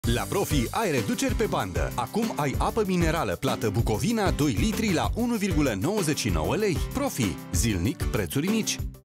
La Profi ai reduceri pe bandă! Acum ai Apă minerală plată Bucovina 2L la un pret special.
La Profi ai reduceri pe bandă! Acum ai Apă minerală plată Bucovina 2L la un pret special. Oferta supusa unor termene si conditii. Profi, zilnic preturi mici.